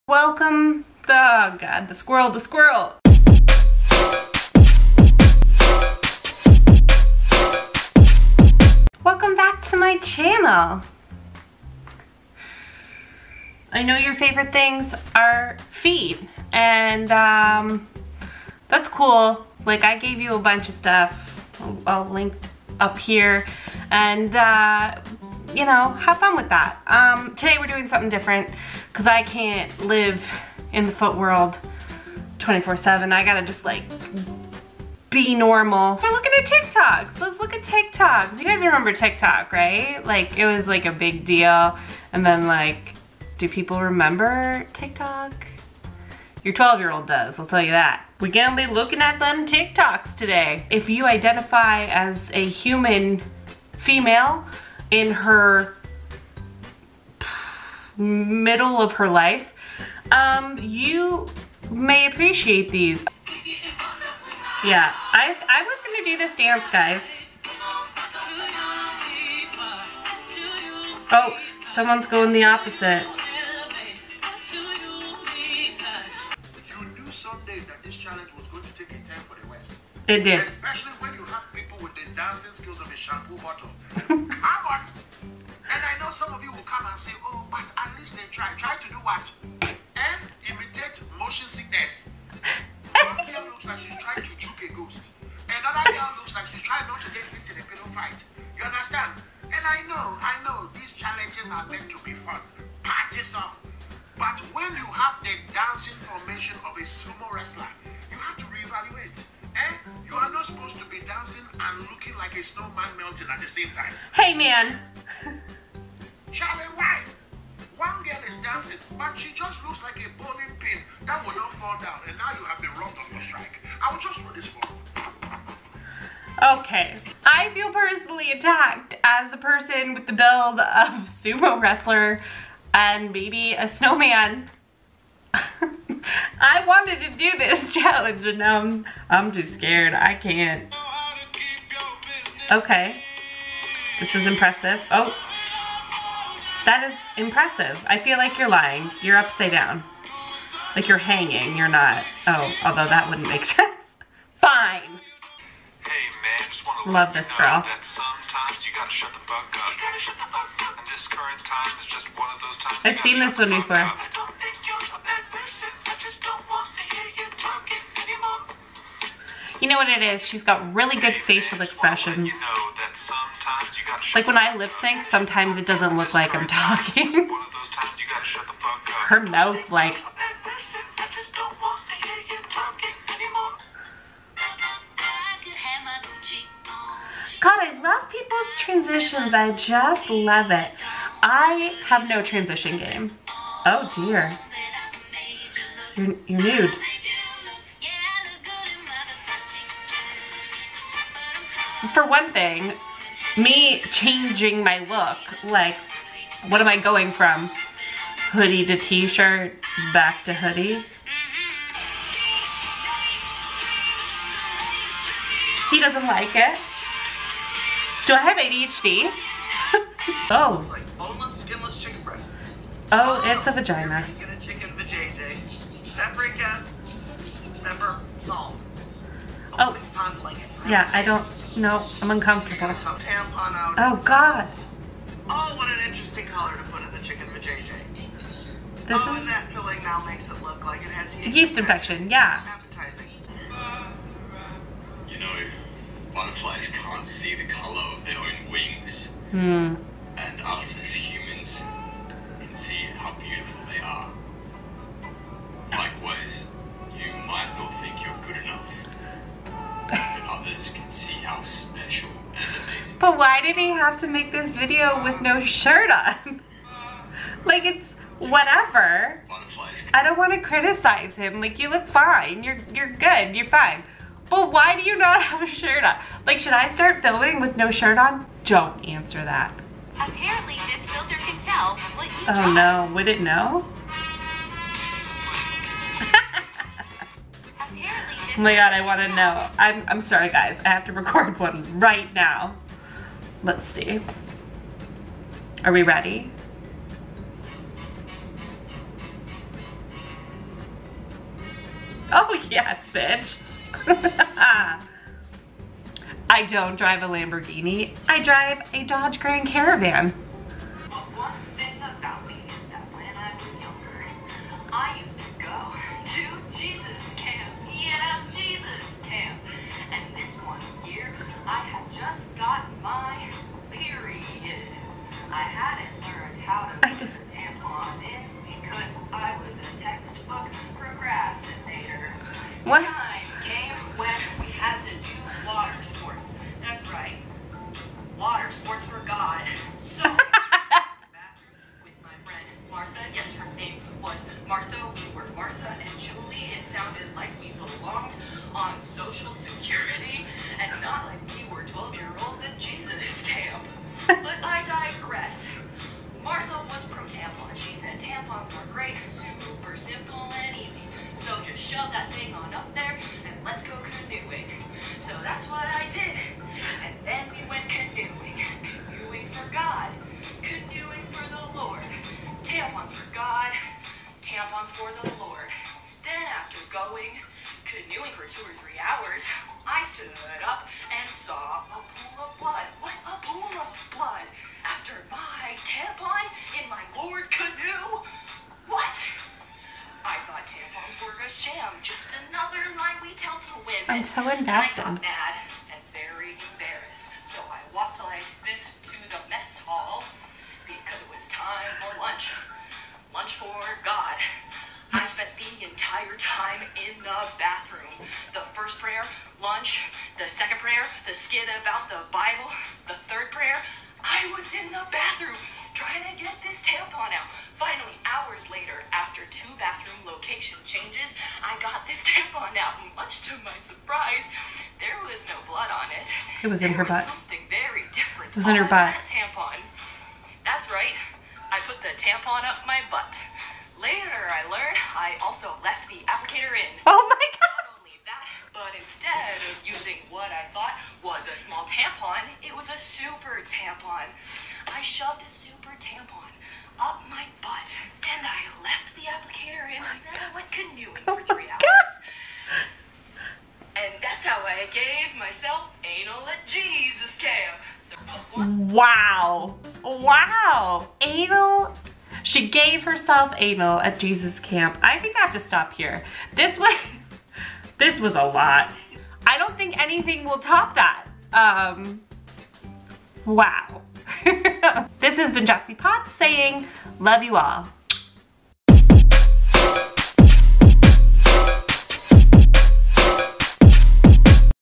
Reacting to Funny TIKTOKS
Taking a break from the footlife to bring you some regular middle-aged lady shizz. Enjoy the TikToks I enjoy and have a little laugh with me.

00:00 Introduction
01:03 #1
02:21 #2
02:35 #3
03:03 #5
03:32 #6
03:43 #7
04:03 #8
04:40 #9
05:11 #10
07:38 Conclusion